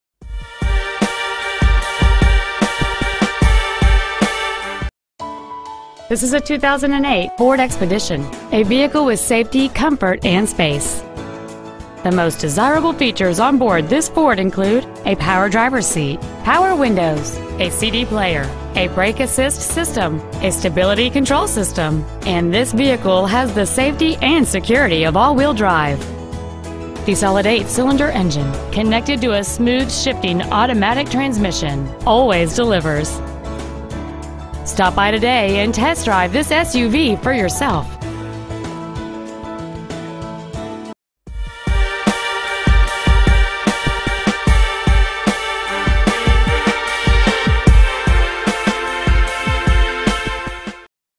Used 2008 Ford Expedition Auburn WA
Year: 2008
 Make: Ford
 Model: Expedition
 Engine: 5.4L V8 SOHC
 Trans.: Automatic
 Exterior: Red

 Interior: Beige

 2008 Ford Expedition Auburn WA